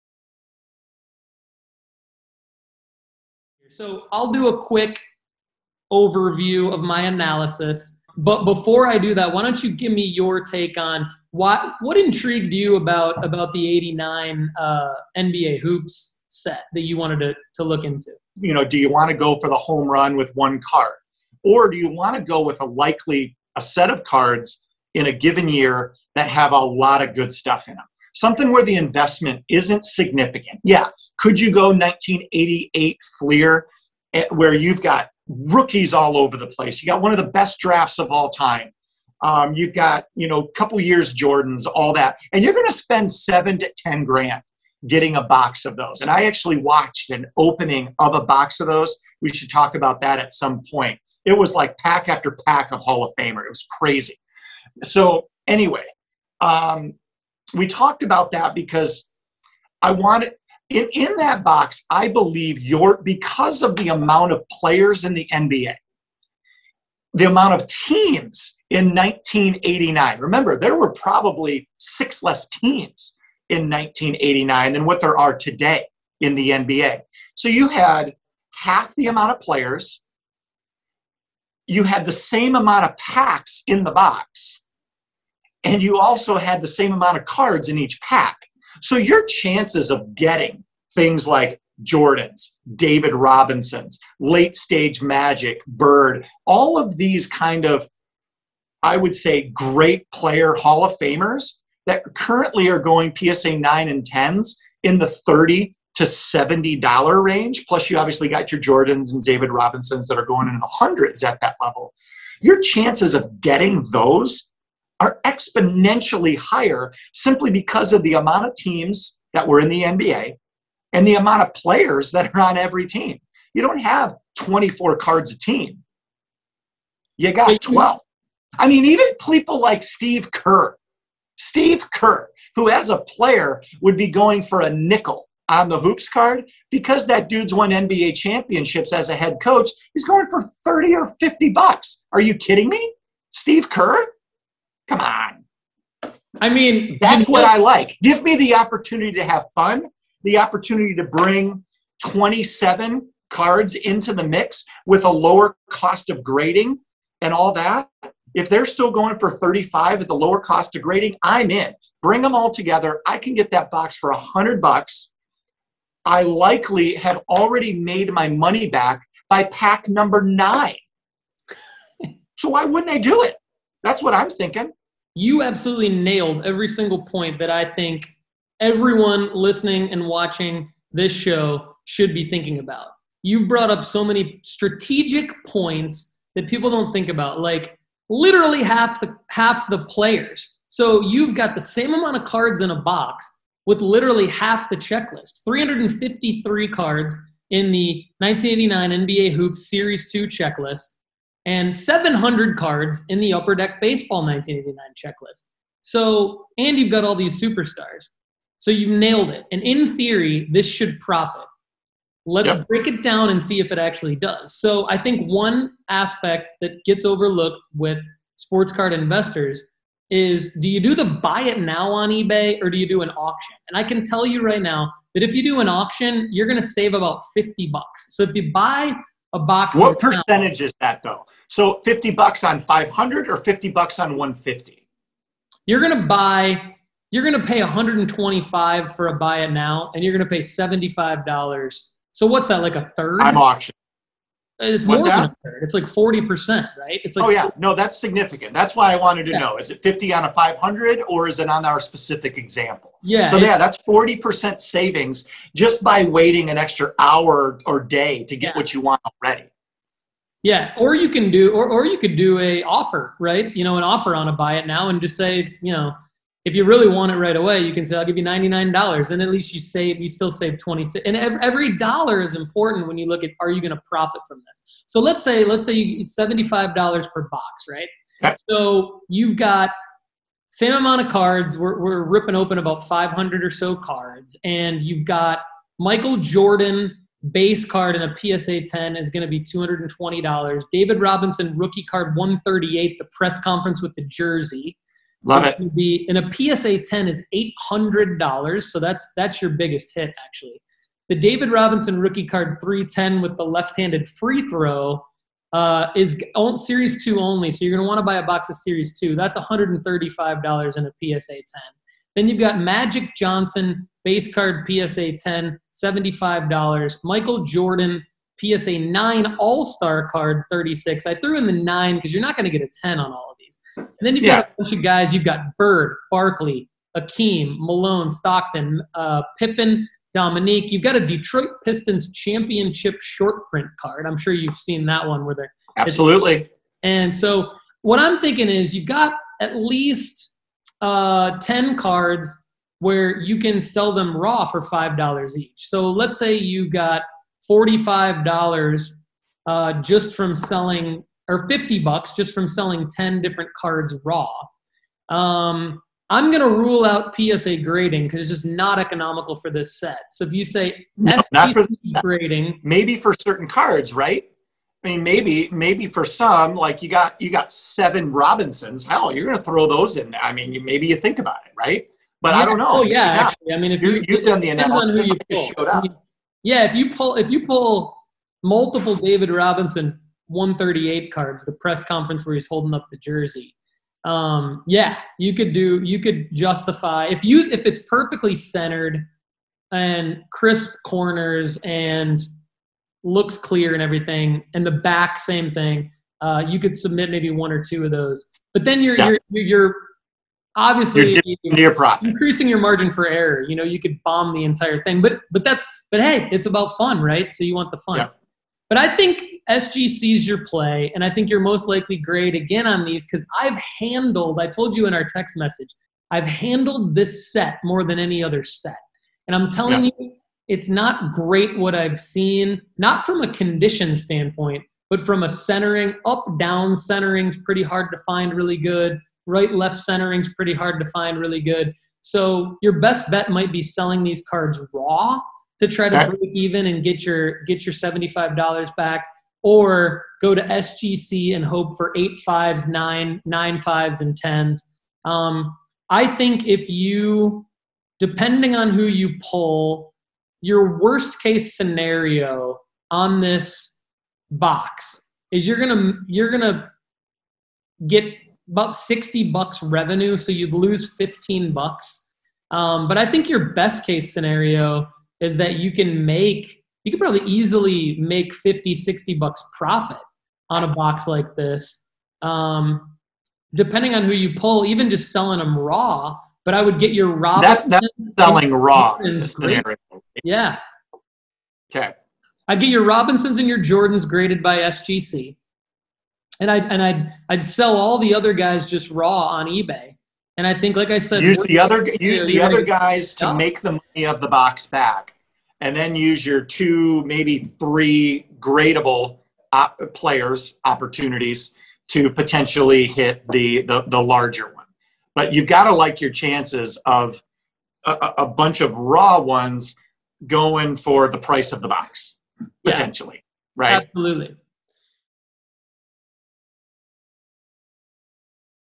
Is A Sealed Box of 1989 NBA Hoops Cards A Good Investment?
At first glance, many eBay listings look like great deals, or at least reasonable investments. But it's time to take a deep dive into specific products to really see if they project to be solid investments.

Vintage products typically don't come cheap anymore, but one product we all likely can afford, is a sealed box of 1989 NBA Hoops.

But is it a good investment? Or just good fun?

Murph and I dig deep in this clip.